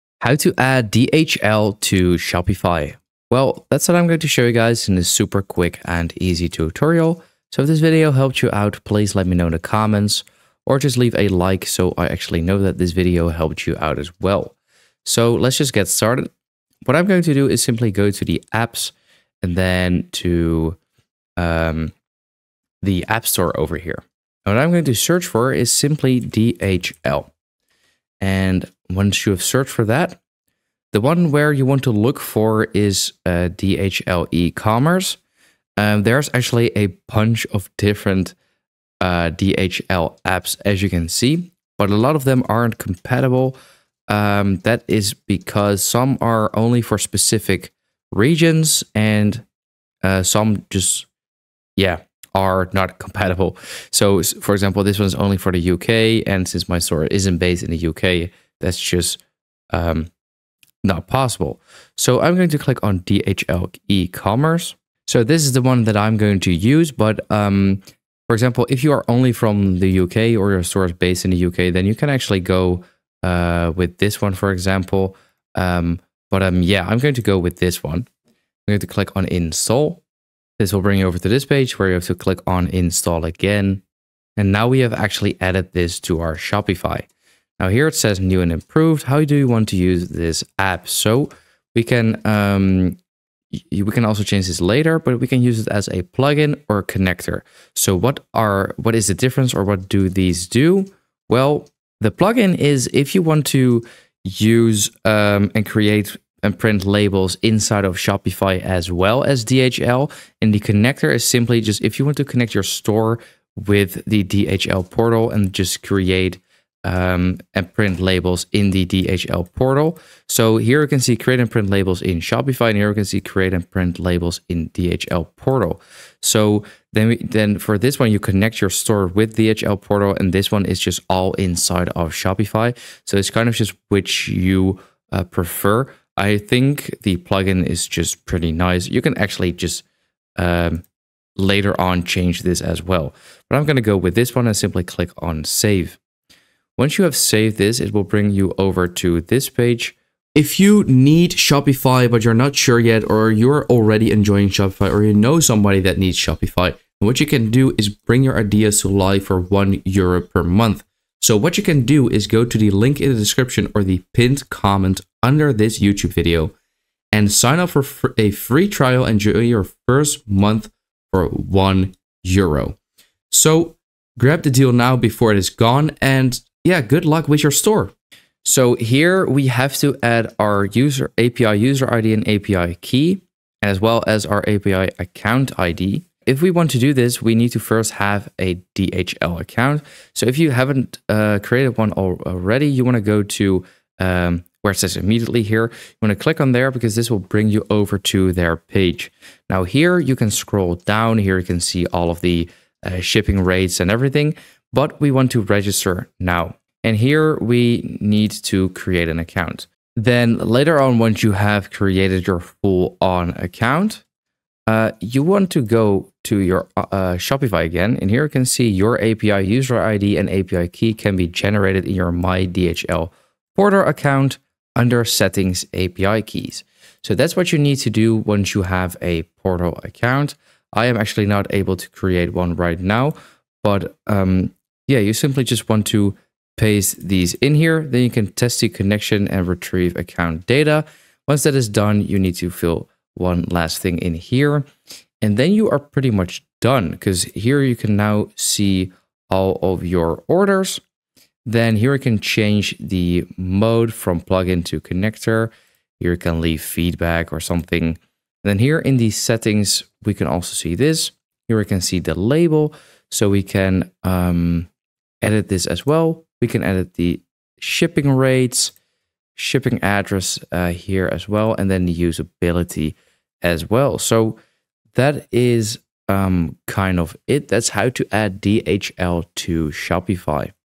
How to Add DHL to Shopify (2025)
In this video I show you how to Add DHL to Shopify 2025

Do you want to know how to connect dhl to shopify 2025? Then I'll show you shopify dhl integration easily.

This is a step by step Shopify tutorial on how to use dhl shopify app